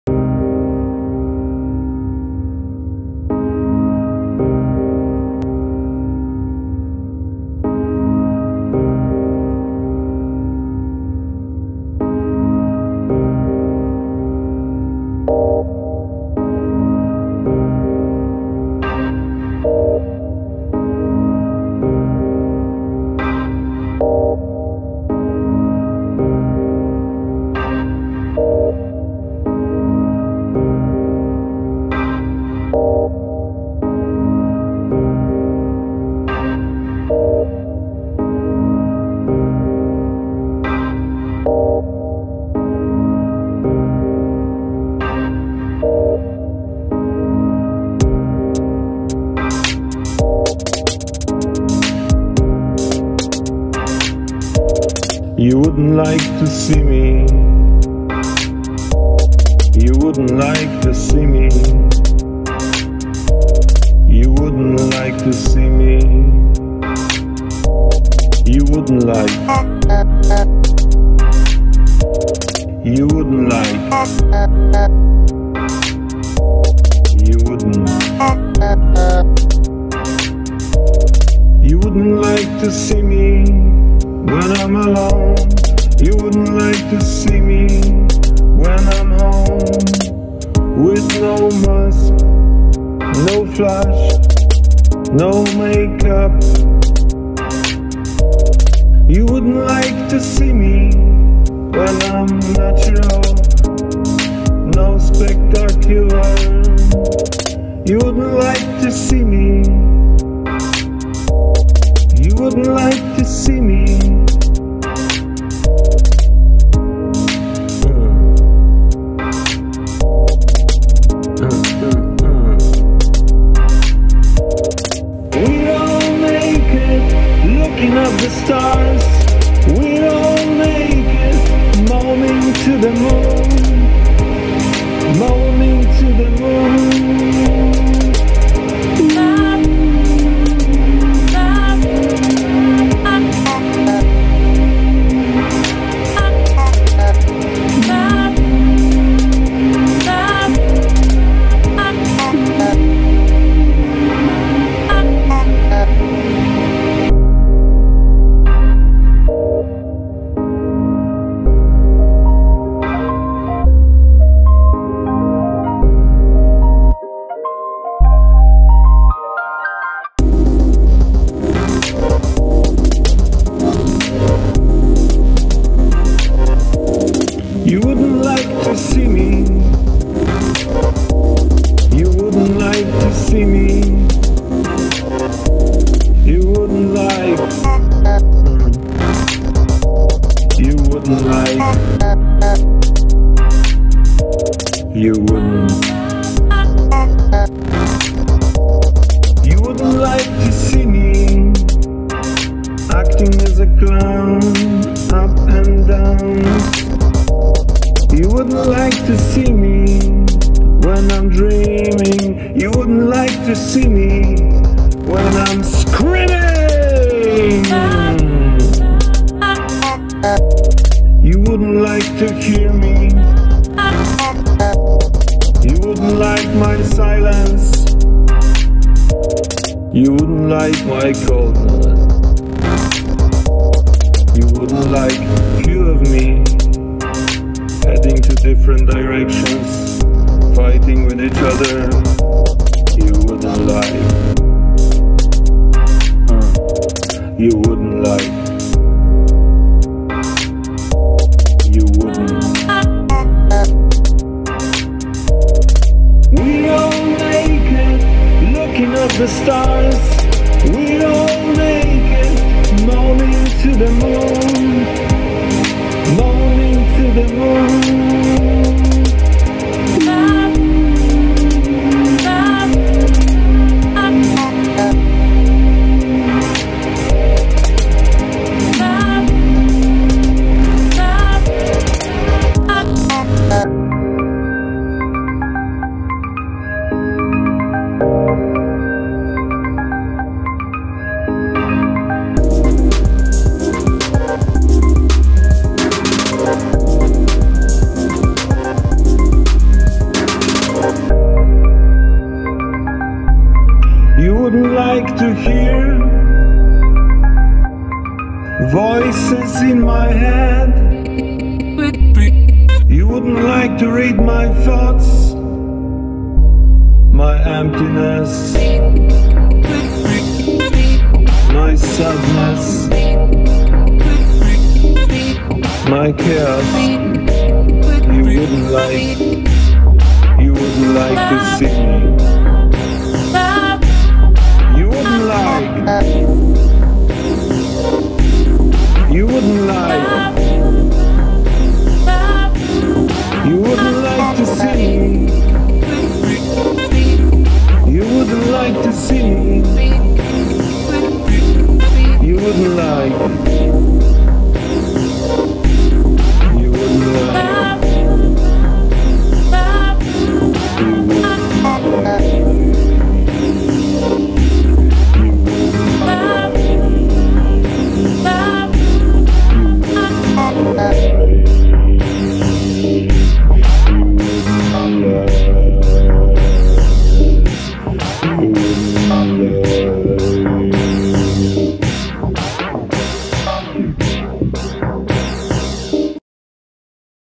You Wouldn’t Like To See Me
That kind of mood, when you think that people create themselves into different personalities to make other people happy. Therefor they are loosing their inner self and are acting constantly.